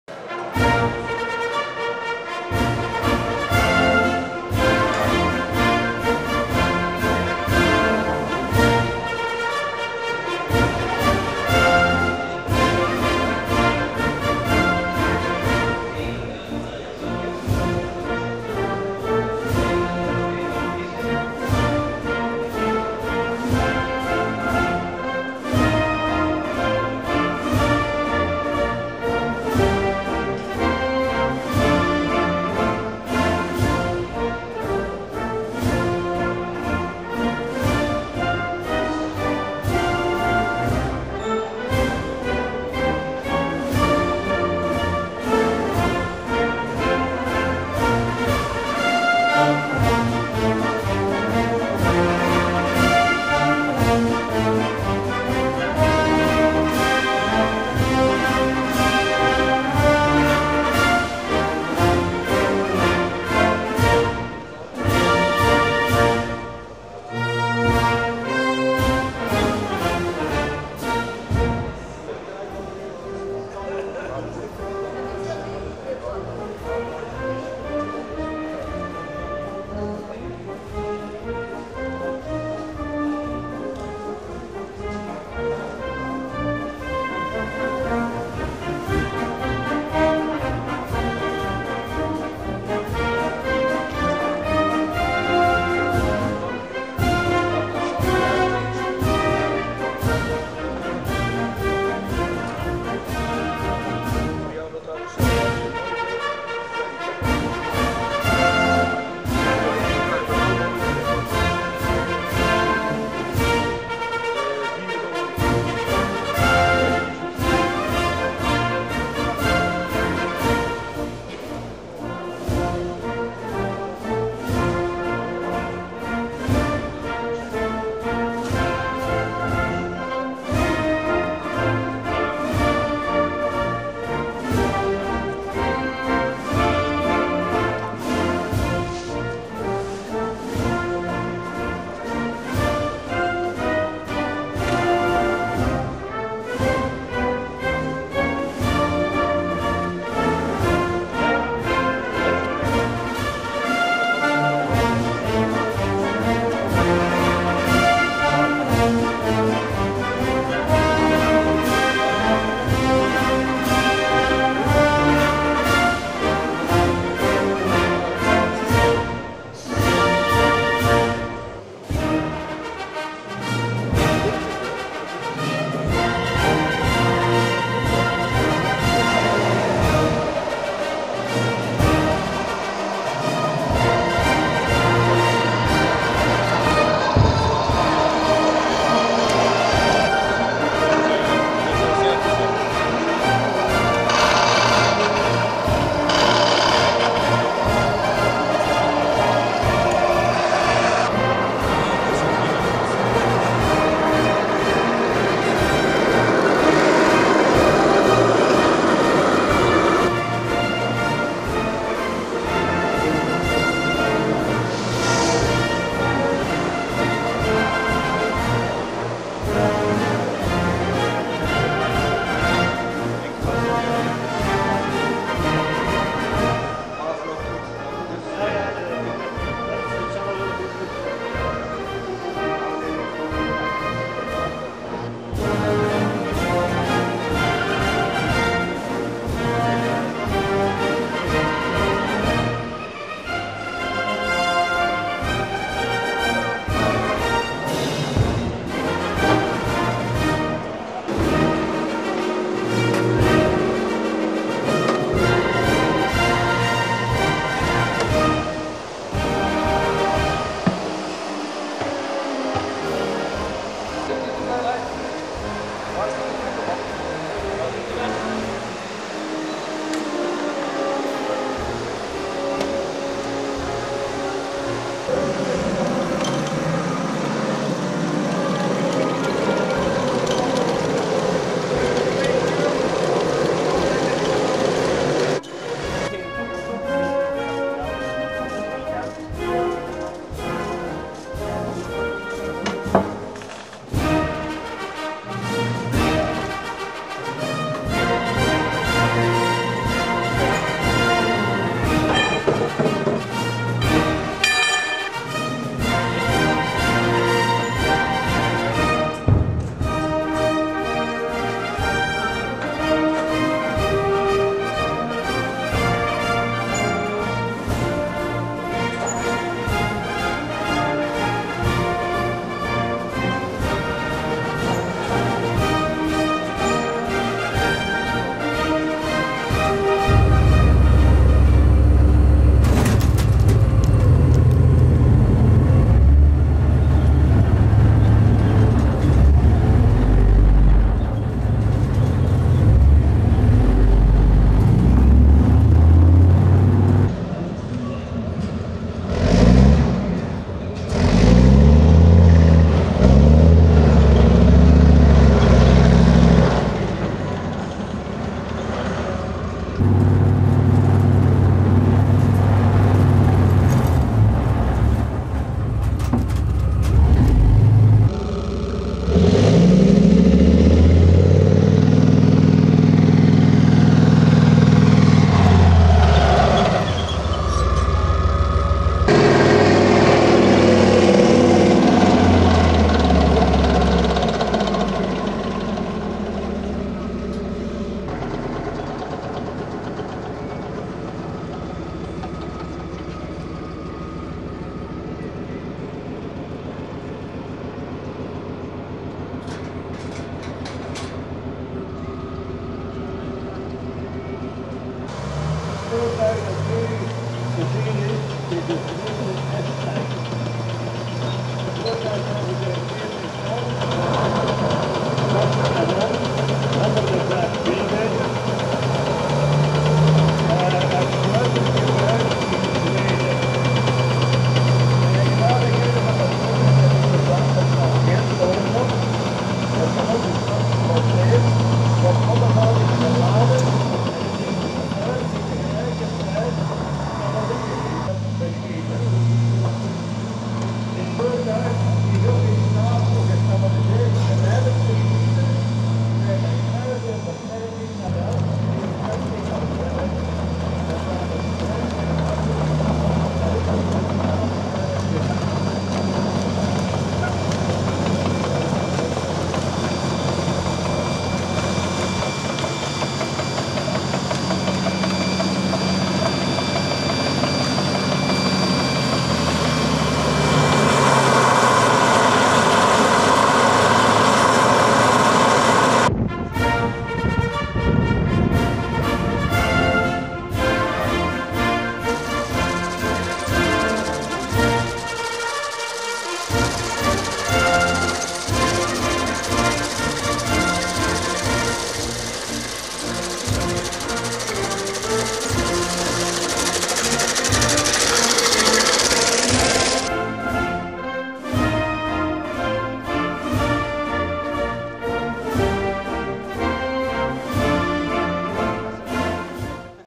Derde modelbouw beurs
Voor de derde keer werd er op Gunfire een modelbouw beurs georganiseerd.